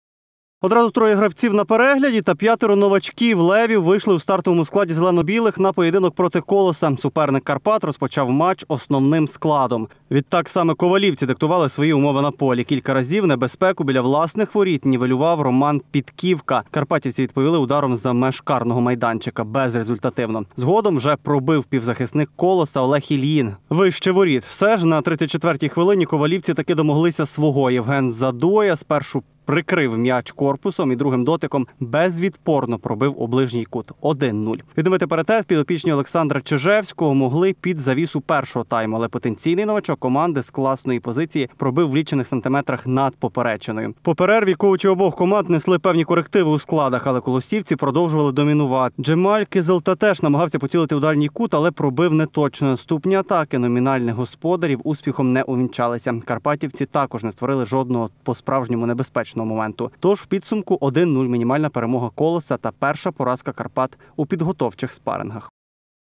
Футбольний матч "Карпати":"Колос": який результат?
Карпати продовжують підготовку до нового сезону насичену контрольними спарингами. На цей раз "зелено-білі" зіграли товариський поєдинок із дебютантом Прем'єр-ліги "Колосом" із Ковалівки. Як завершився матч дивимося в огляді